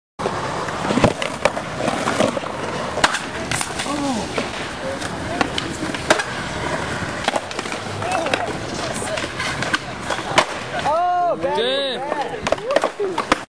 Ann Arbor Quick Clip
A few clips from the new Ann Arbor skate park. Featuring Nate Kontur and Joey Sinay. Filmed by Brendon Kontur